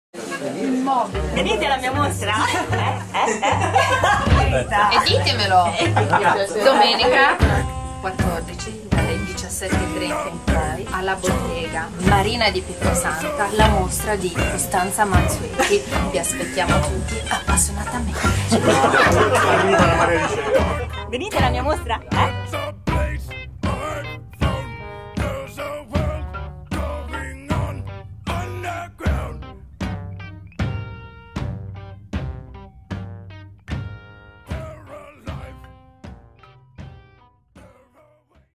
Spot divertente Stand Still mostra di Costanza Mansueti
Cesare Tomei, durante la cena fatta da LABottega di Marina di Pietrasanta ha fatto due riprese divertenti... e noi ci facciamo un piccolo spot!